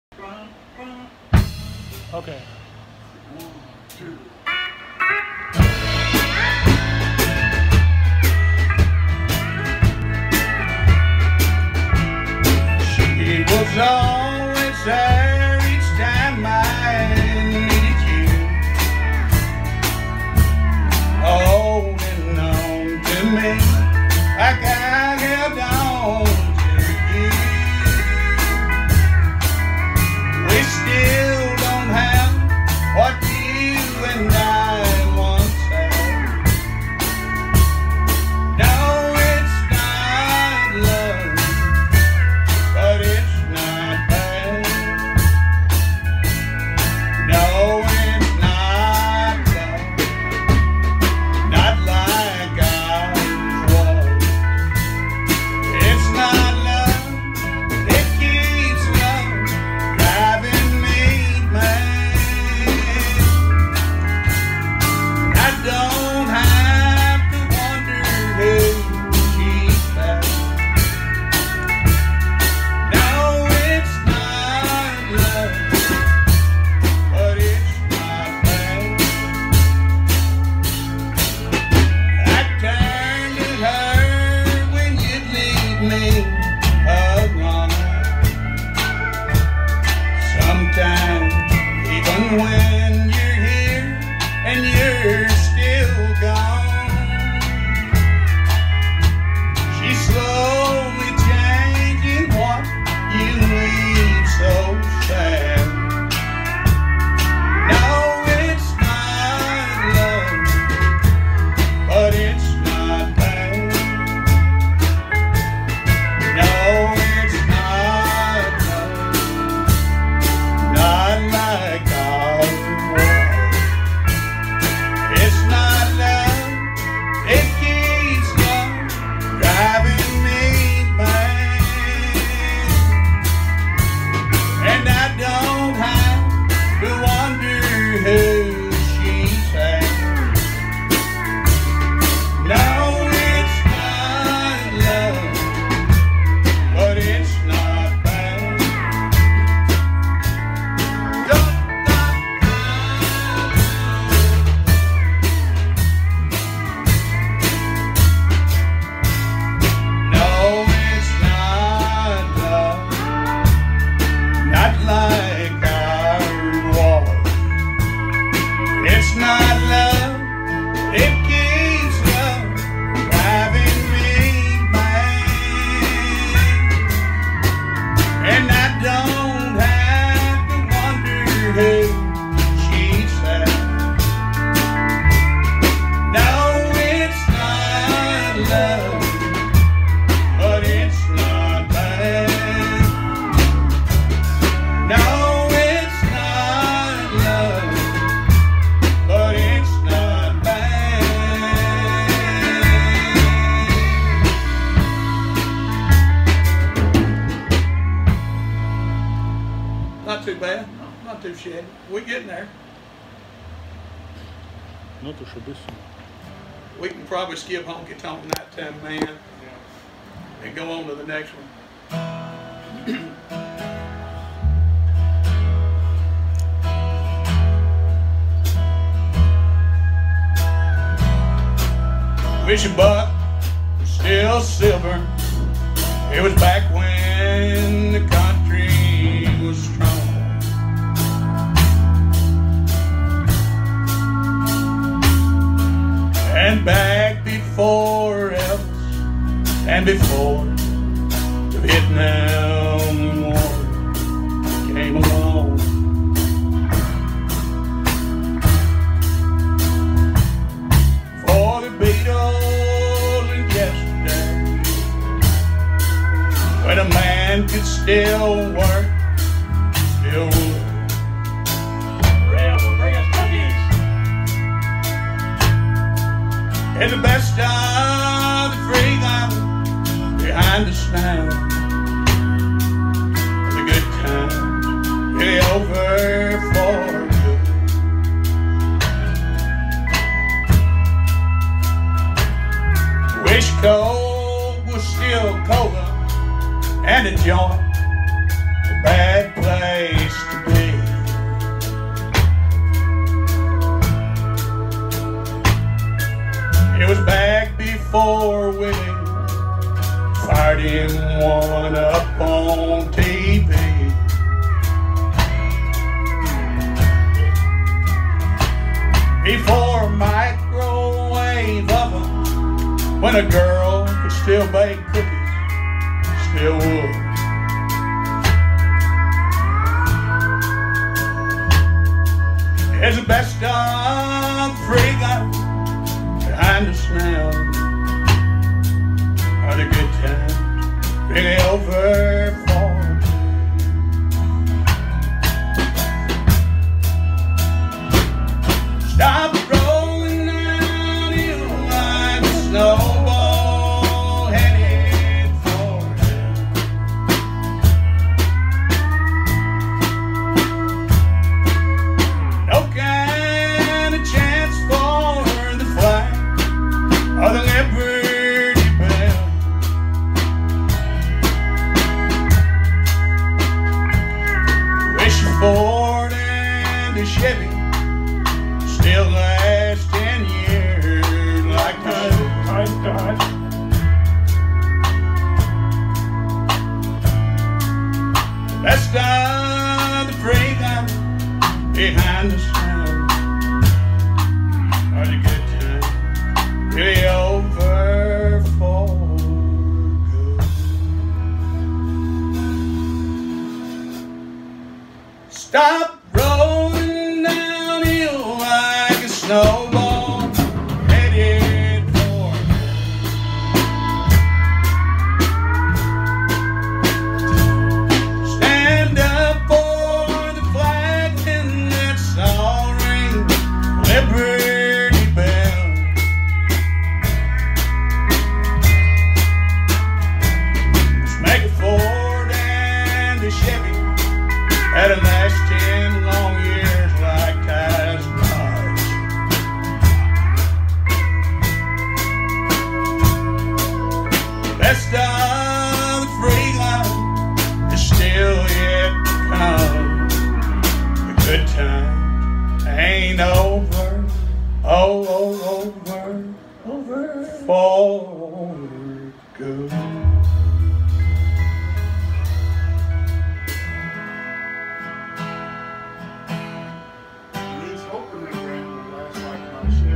Ronnie Moore
Ronnie and the guys rehearsing some Hag stuff with Ray, Ty and Damir